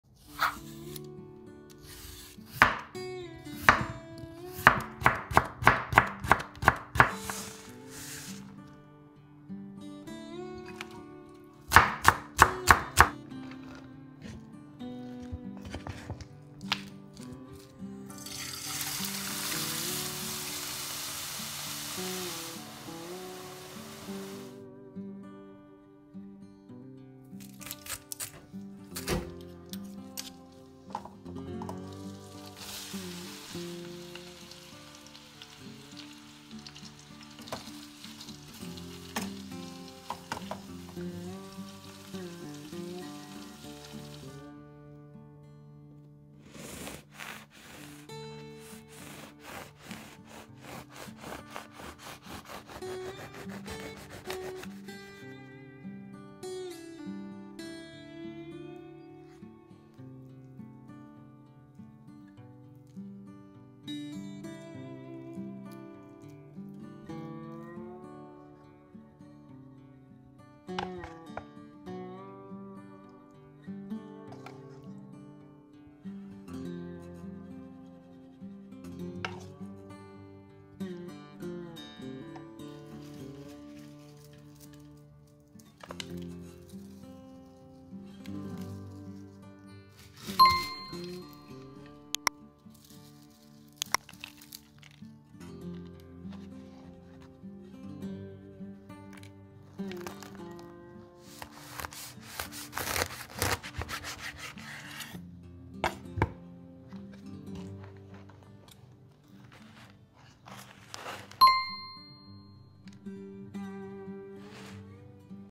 Crunchy, Crispy, Tender and Creamy, All you want in this Prawn Sandwich!
All you want in this Prawn Sandwich with Croissant. Crunchy, Crispy, Tender and Creamy!